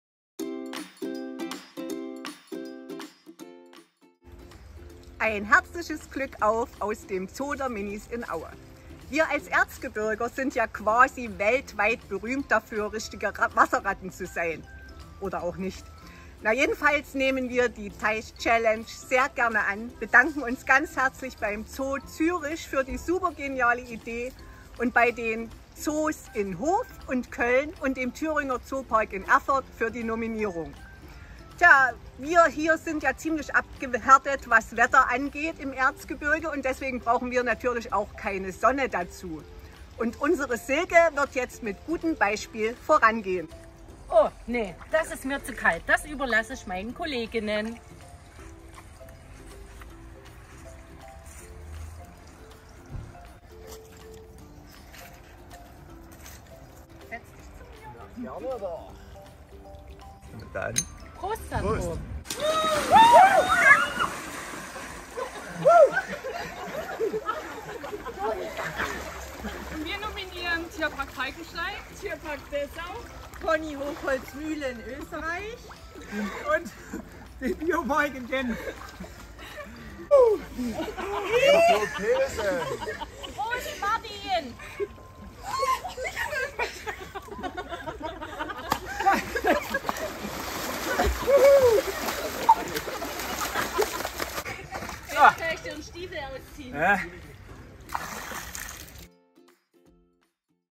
2022/130 - 400 tierische Zwerge sind in Aue zuhause | Teich-Challenge
Der Auer Zoo der Minis hat aus Erfurt, Köln und Hof die Nominierung zur Tech-Challenge erhalten. Die Idee dazu ist im Zoo Zürich geboren worden.